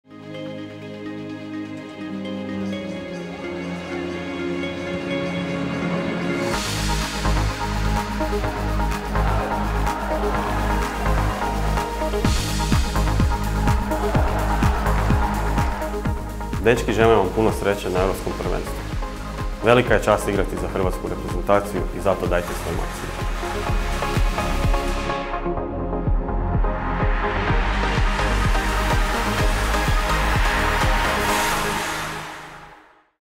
HNTV najava: Poziv na U-17 Euro - Niko Kranjčar
Pred hrvatskom U-17 reprezentacijom nastup je na Europskom prvenstvu koje će se održati u Hrvatskoj, a podršku mladim Vatrenima dao je hrvatski nogometaš te ambasador Eura Niko Kranjčar.